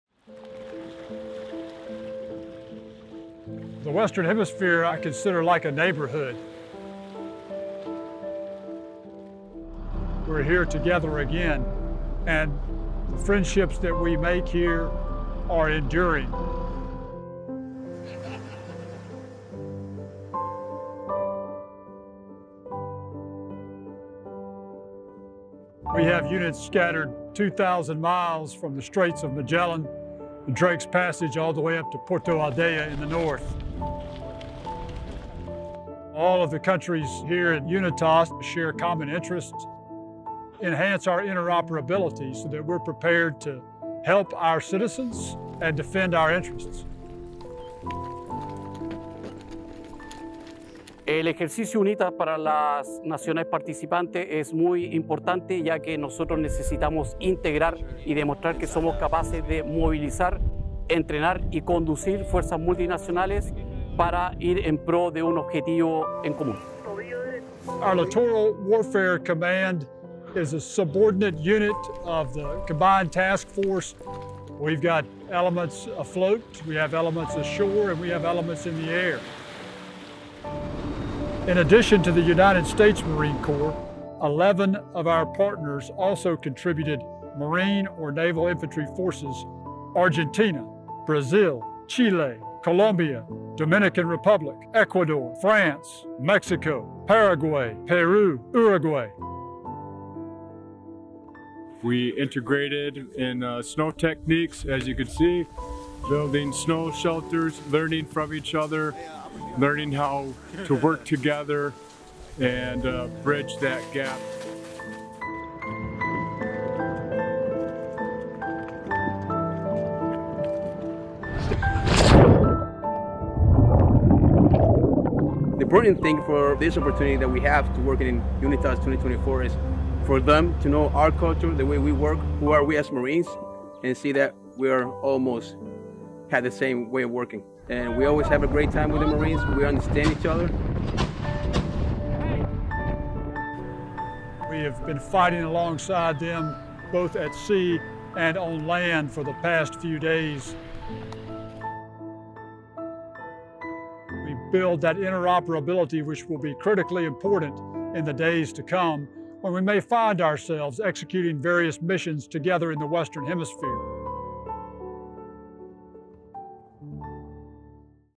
UNITAS LXV in Chile with U.S. Marines and Allied and Partner Nations
Allied and partnered nations conduct multilateral training during UNITAS LXV in Chile, Sept. 2 - 12, 2024. UNITAS, hosted by Chile this year, is the world’s longest-running annual multinational maritime exercise that focuses on enhancing interoperability among multiple nations and joint forces during littoral and amphibious operations in order to build on existing regional partnerships and create new enduring relationships that promote peace, stability, and prosperity in the U.S. Southern Command’s area of responsibility. UNITAS, which is Latin for “unity,” was conceived in 1959 and has taken place annually since first conducted in 1960. This year marks the 65th iteration of the world’s longest running annual multinational maritime exercise.

(U.S. Marine Corps video by Lance Cpl. Madisyn Paschal)